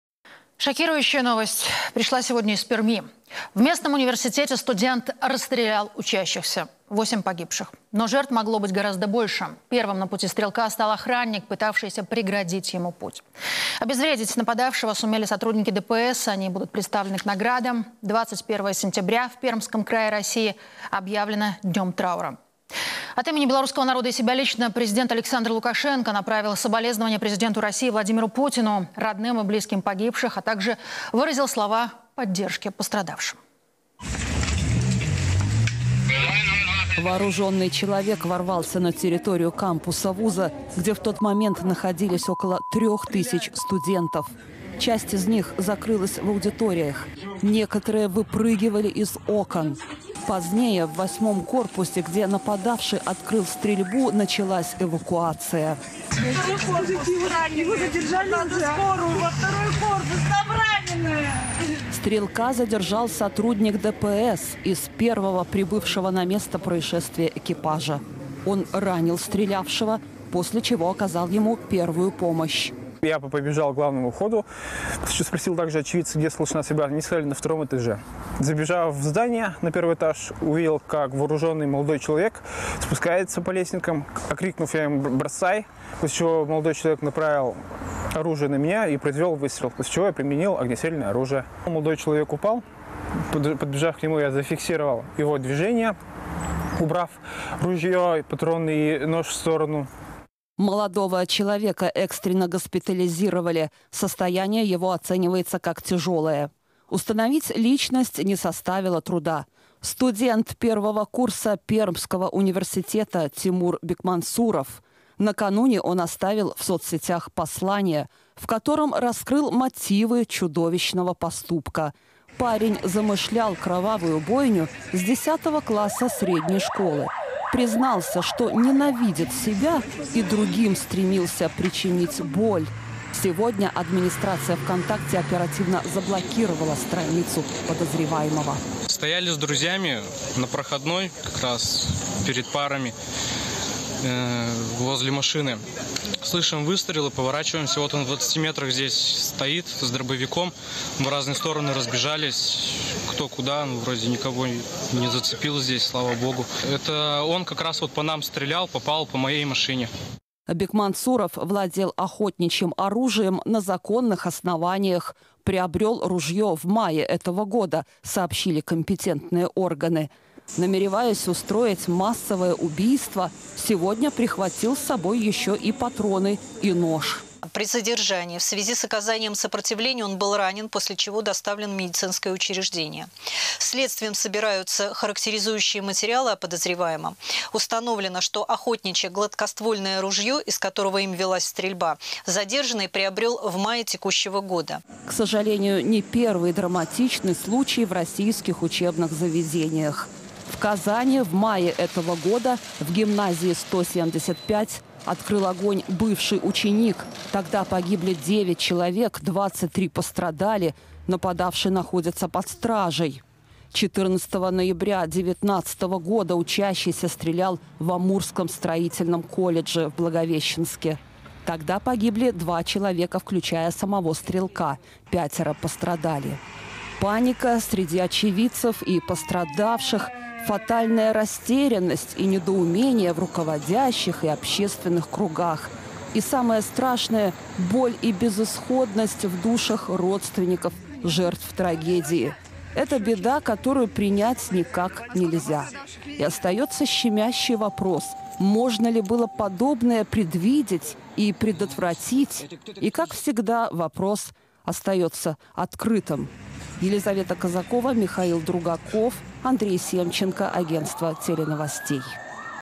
Зачем студент устроил стрельбу в Пермском университете? Панорама
В результате стрельбы в университете в Перми погибли восемь человек. Первой жертвой стрелка стал охранник, пытавшийся преградить ему путь. Обезвредить нападавшего сумели сотрудники ДПС, они будут представлены к наградам. 21-ое сентября  в Пермском крае объявлено днём траура. Подробности — в видео.